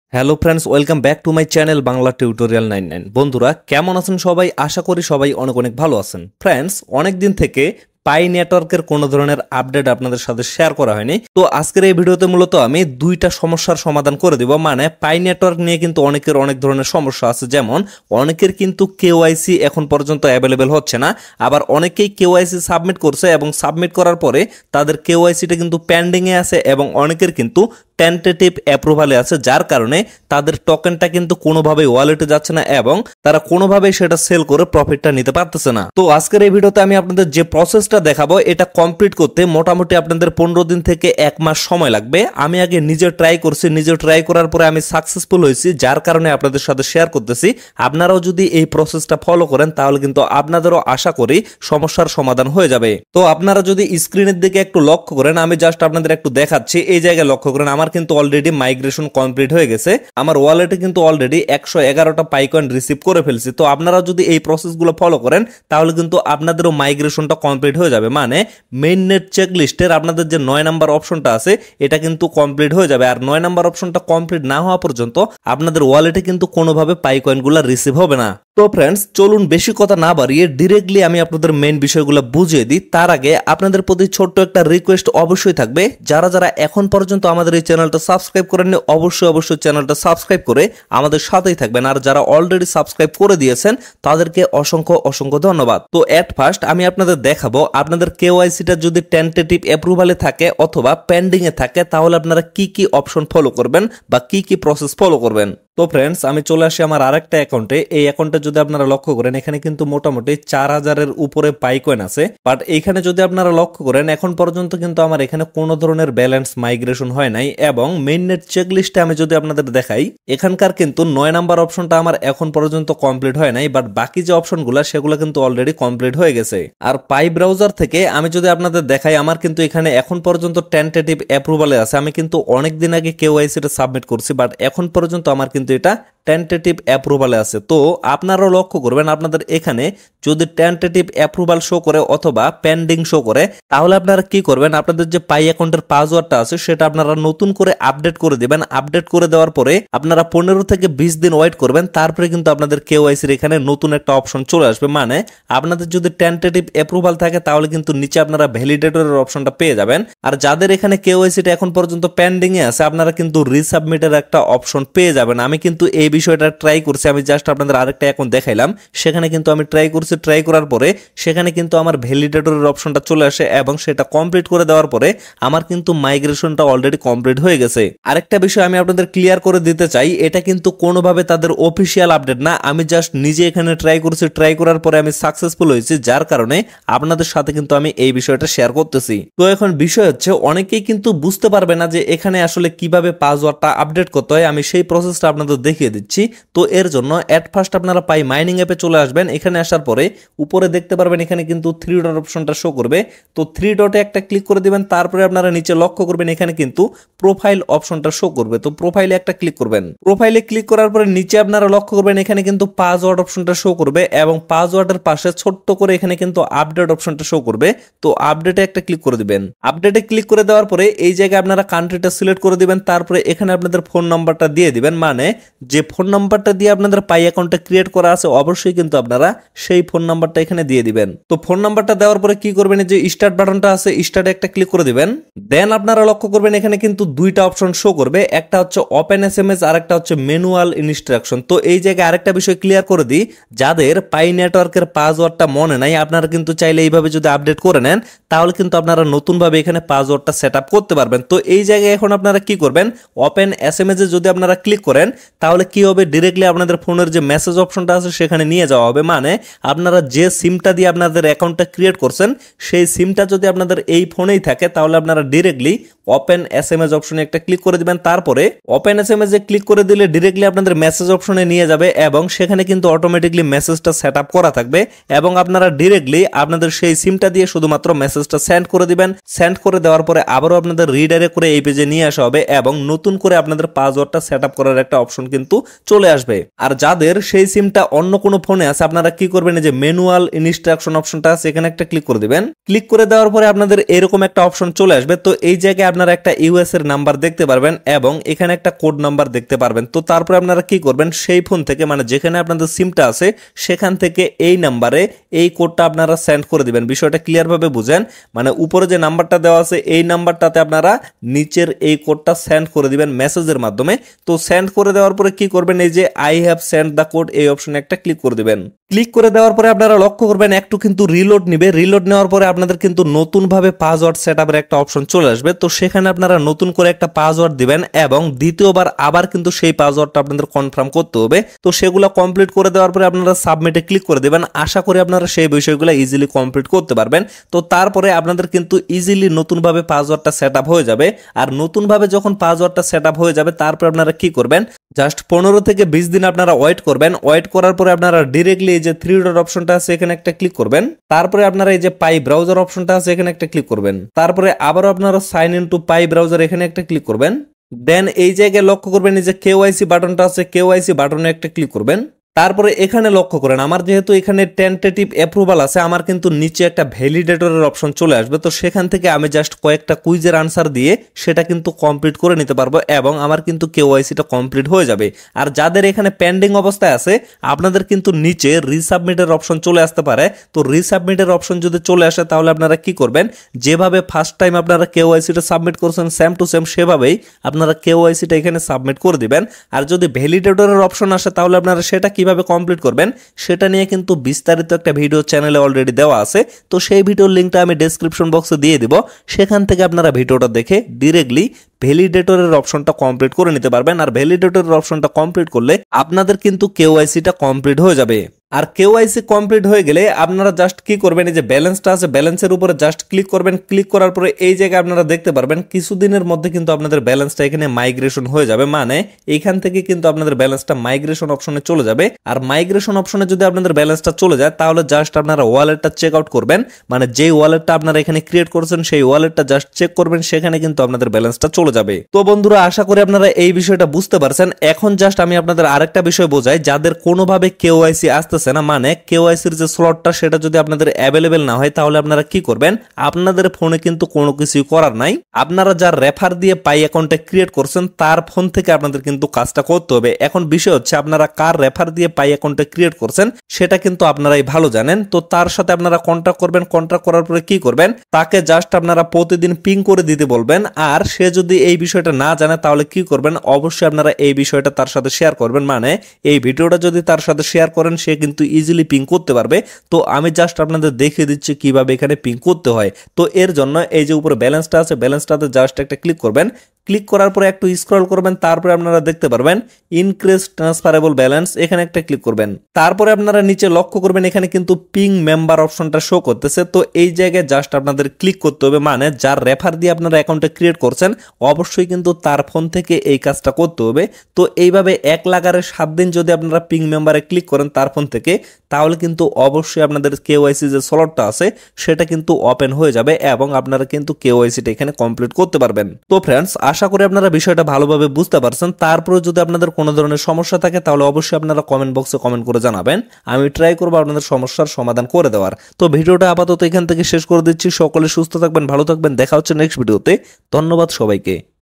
Pi Network KYC Pending | Pi Tentative Approve | Pi Network All Problem Solve | Pi Network New Update
The latest updates from the Pi Network are shown in this video. In this video I have shown you how to do email verification and phone verification and Facebook verification on Pi Network. Those who have not yet created a Pi Network account must create an account very soon if they do not create one later.